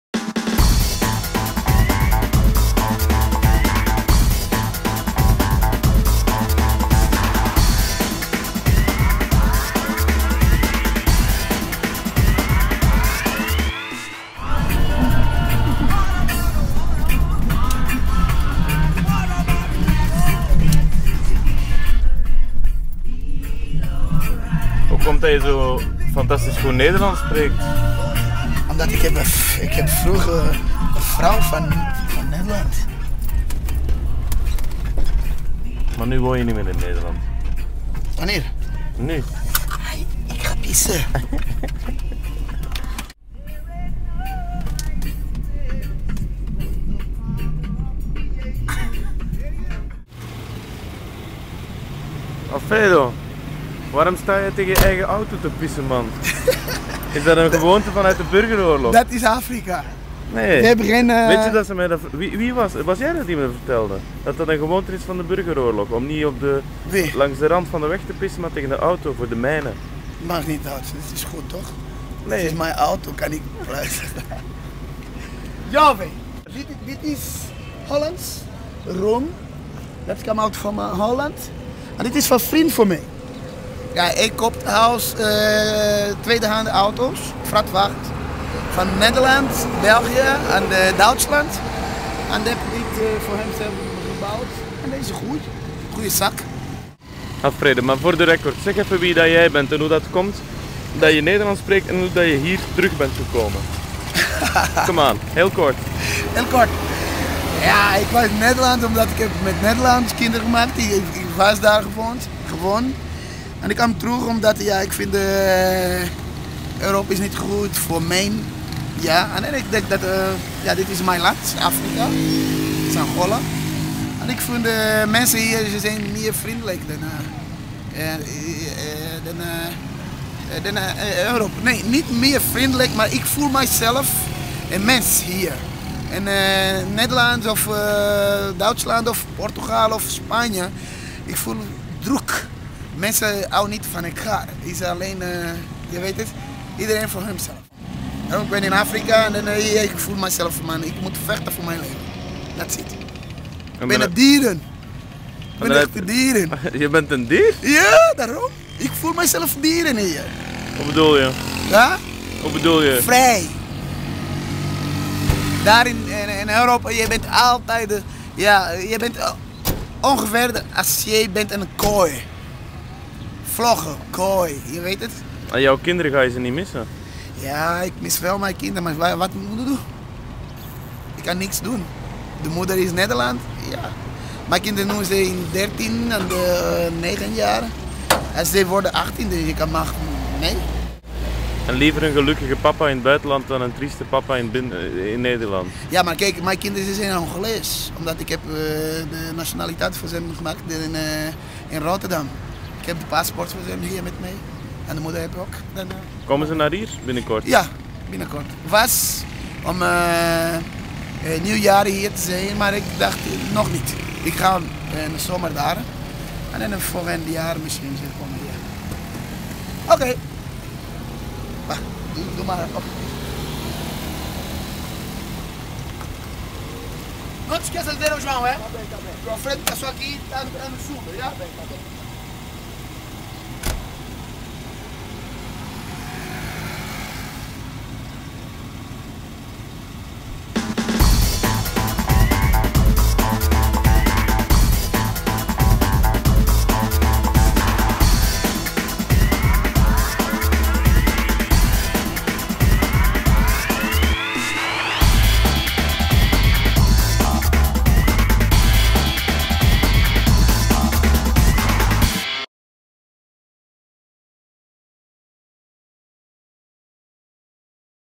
'In Angola voel ik mij vrij als een dier'
Nederlander Alfredo Armada is na jaren in Nederland teruggekeerd naar Angola en legt uit waarom. Dutch guy, Alfredo Armada, moved back to Angola and explains why. (audio from iMovie 7.1.4)